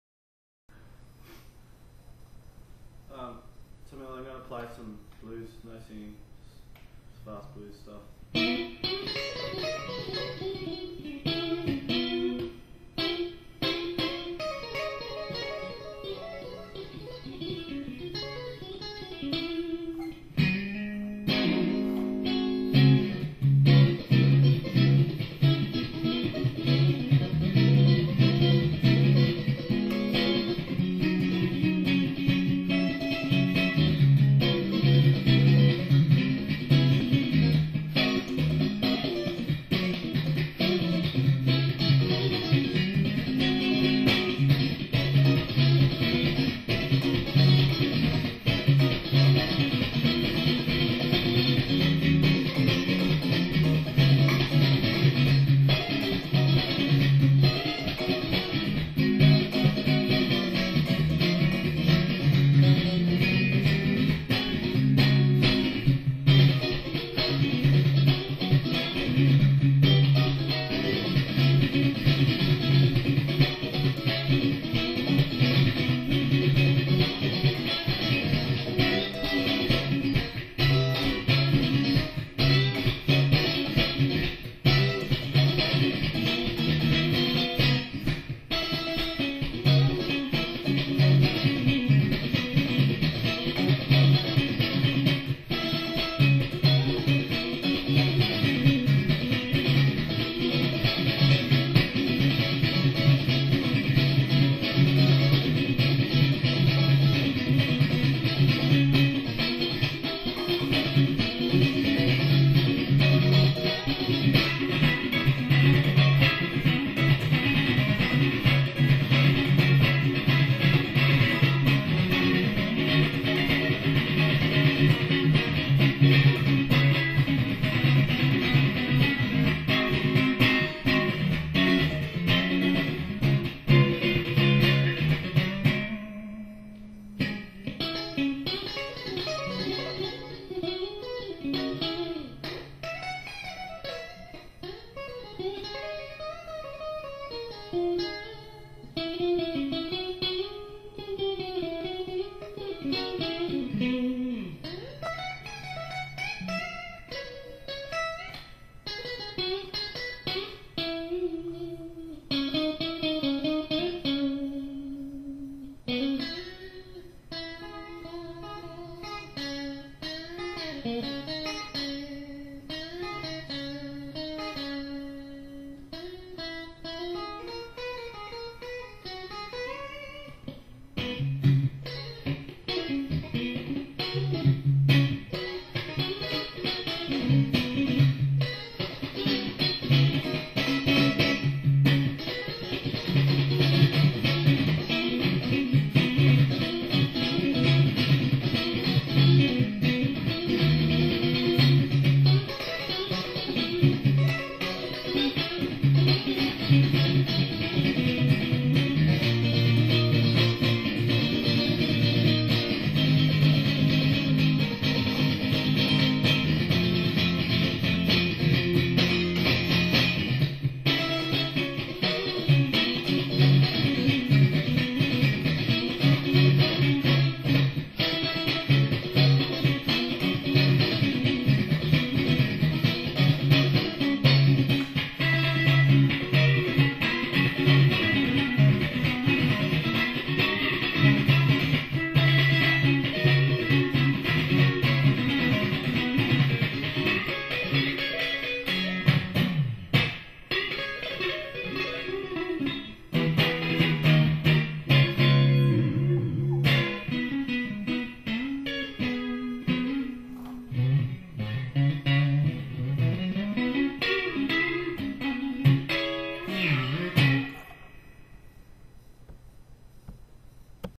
Fast Blues Tom Hurley
Me mucking around with some bluesy stuff.